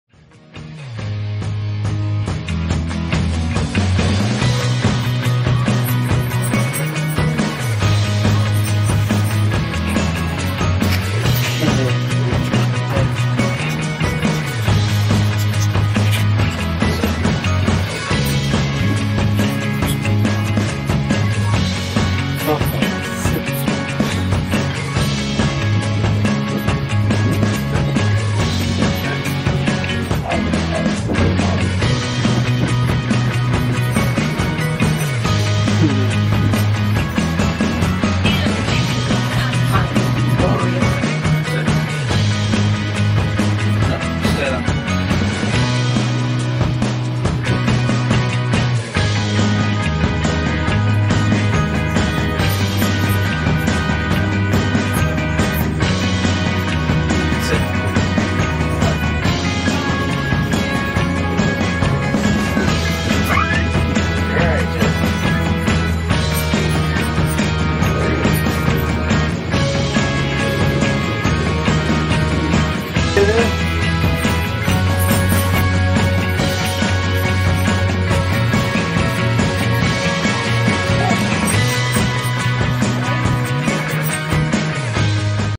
Siberian Husky With Big, Blue Eyes | The Daily Puppy
Blue is a Siberian Husky with big, bright eyes. He is naturally curious and loves to be part of the fun when at the park with his friends!